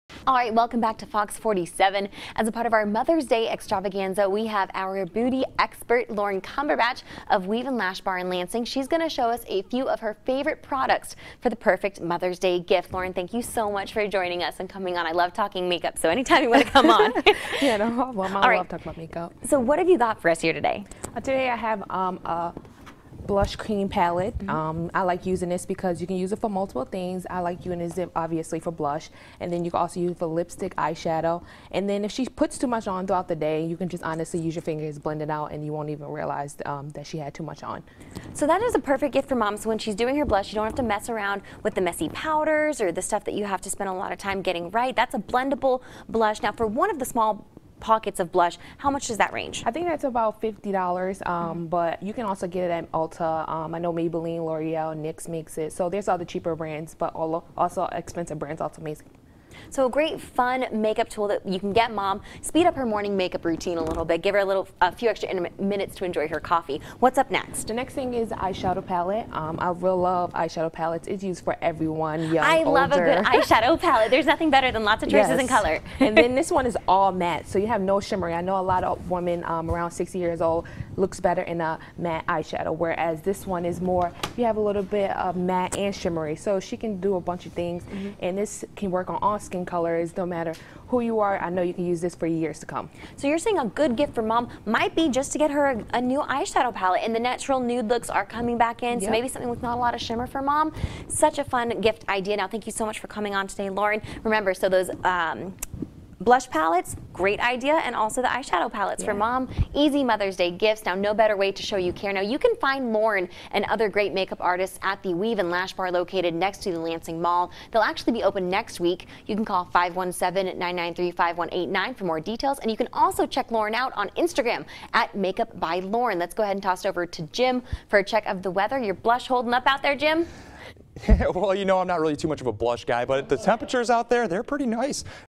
Mom's Day beauty must-haves
Lorin Cumberbatch, Beauty Expert from Weave & Lash Bar, discusses makeup products that make great Mother's Day gifts.